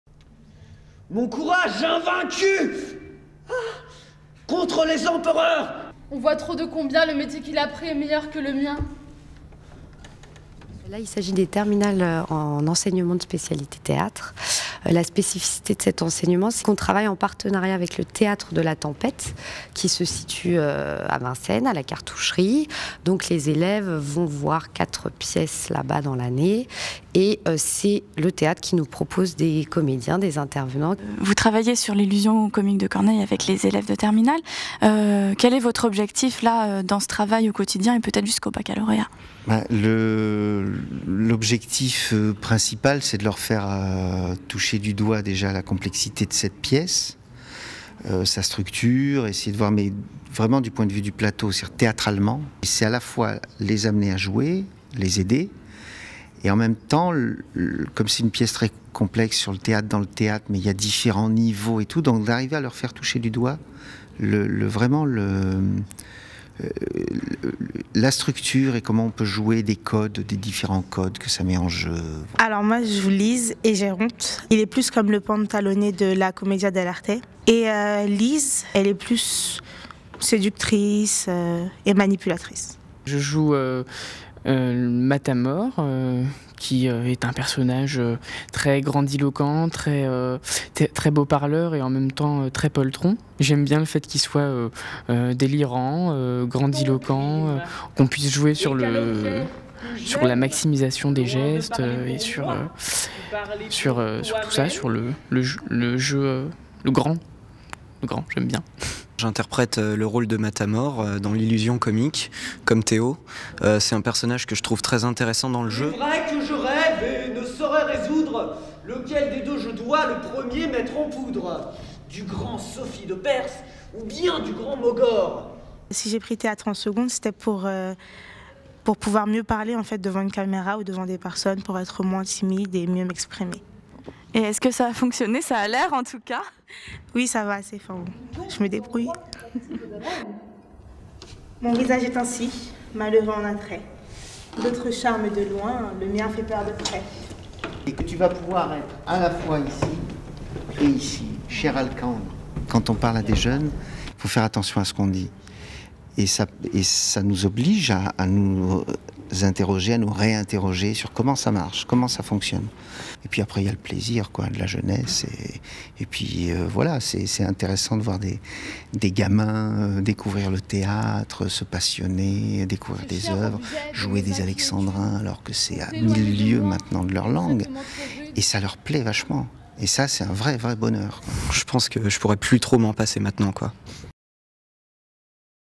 Du théâtre au lycée
En terminale L, spécialité théâtre, les jeunes comédiens du lycée Romain Rolland ont travaillé sur L'illusion comique de Corneille. Dirigés par leur enseignante et un intervenant du Théâtre de la tempête, ils ont appris à apprivoiser les vers.